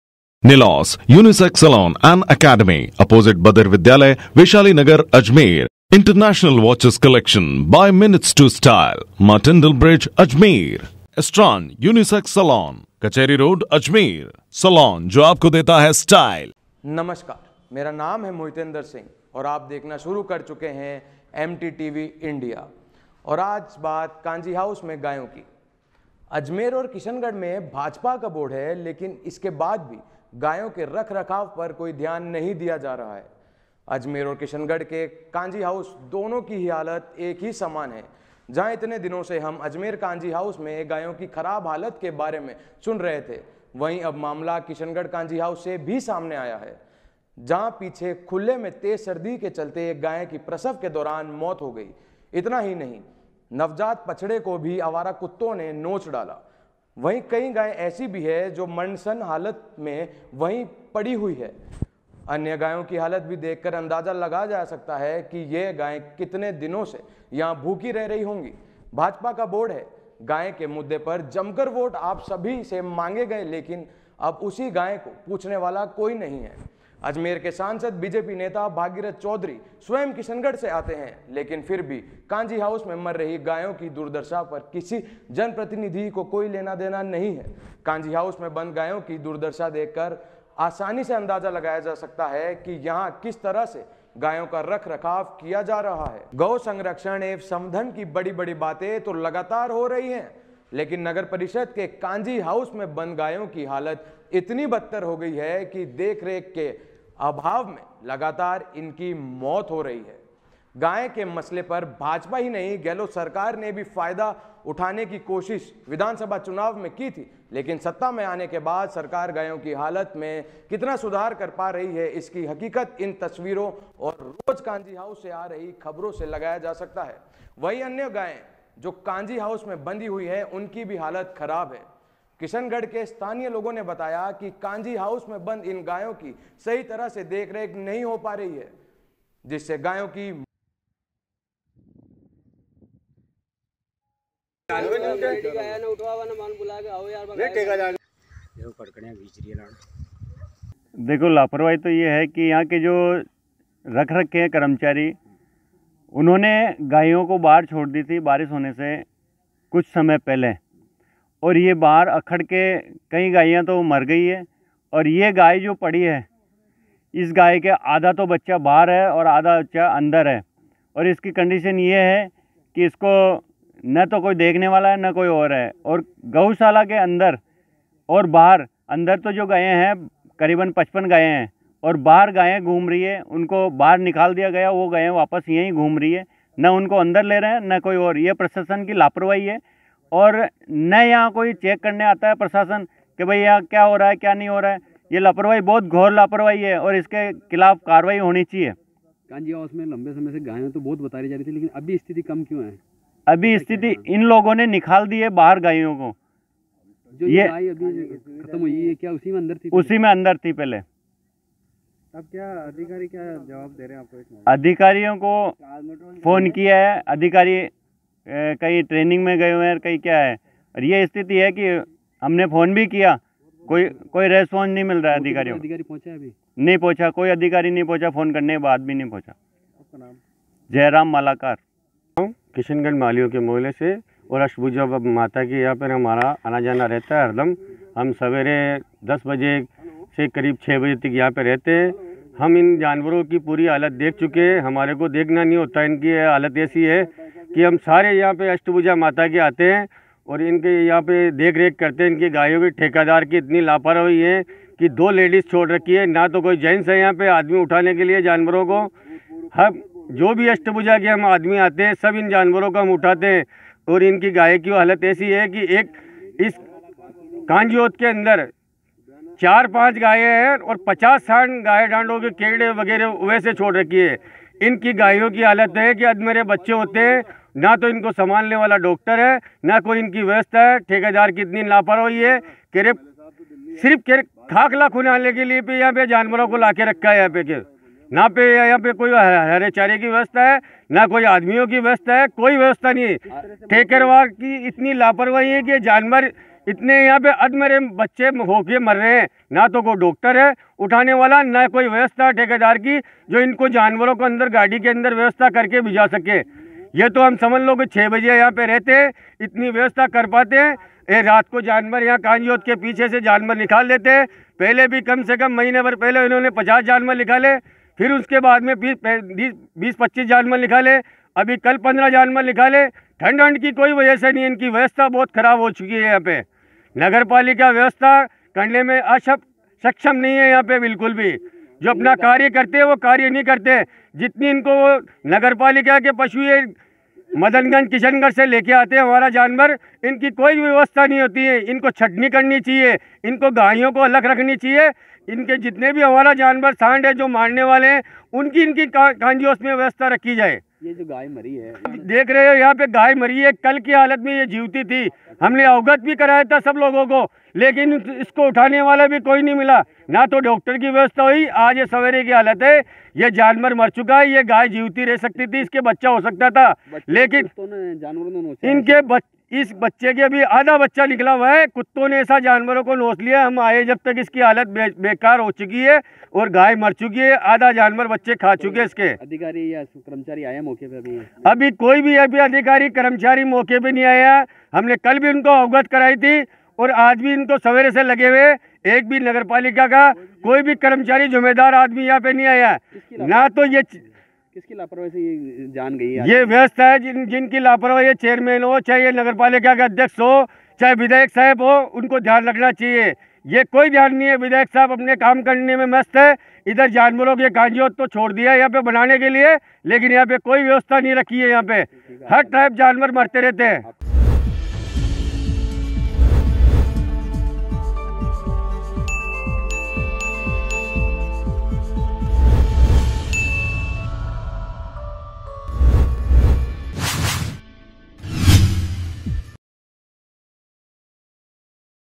Kishangarh | अधिकारियों और कर्मचारियो की लापरवाही के चलते गोवंश की हो रही है यह हालत | MTTV INDIA
किशनगढ़ नगर परिषद के कांजी हाउस में गाय की हालत बदतर ,भाजपा का बोर्ड होने के बावजूद लगातार हो रही है गायो की मौत,तेज सर्दी के चलते प्रसव के दौरान हुई गाय की मौत ,परिषद के कांजी हाउस के बाहर तड़प रही है गाये,अधिकारियों और कर्मचारियो की लापरवाही के चलते गोवंश की हो रही है यह हालत ,क्षेत्रवासियों का आरोप कांजी हाउस से छोड़ी गई गाय की हुई मौत ,जिम्मेदार अधिकारी और जनप्रतिनिधियो ने मूंद रखी है आँखे,गाय के हितों की बाते करने वाले संगठनों ने भी साध रखी है चुपी